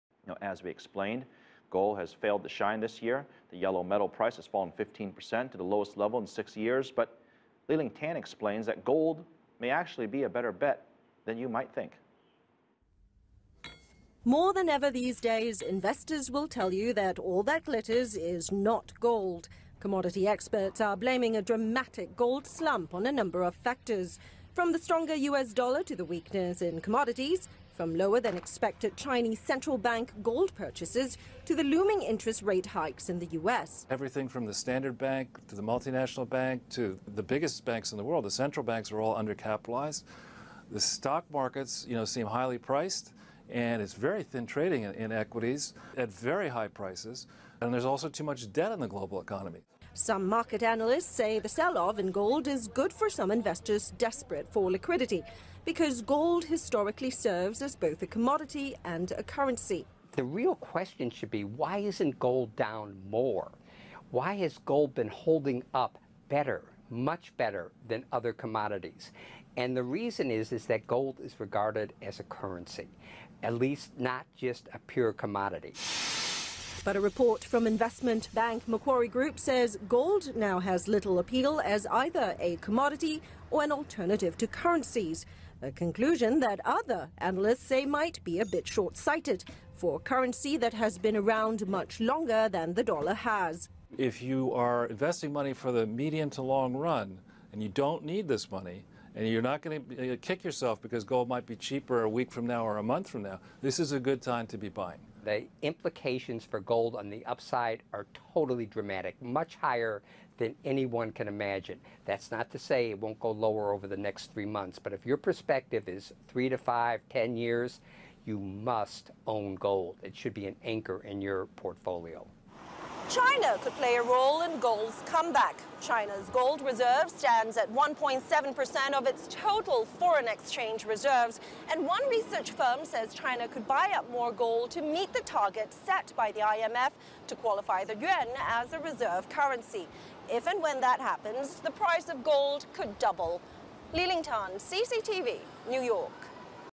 Global gold prices have dropped
The price of gold has continued to drop, hitting a 5-year low in the past week.
To put all this into perspective, our New York correspondent, CCTV America's Liling Tan filed this report.